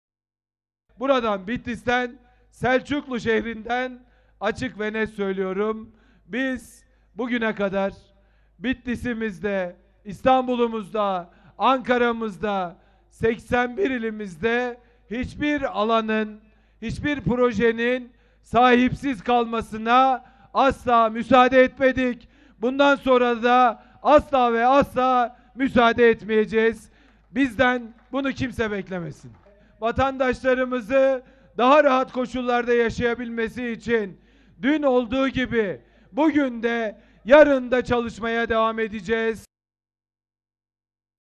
Bakan Kurum'dan ''proje'' açıklaması
Çevre, Şehircilik ve İklim Değişikliği Bakanı Murat Kurum, Bitlis’te Temel Atma ve Toplu Açılış Töreni'nde konuştu. Kurum, ''Biz bugüne kadar 81 ilimizde hiçbir projenin sahipsiz kalmasına asla müsaade etmedik'' ifadelerini kullandı.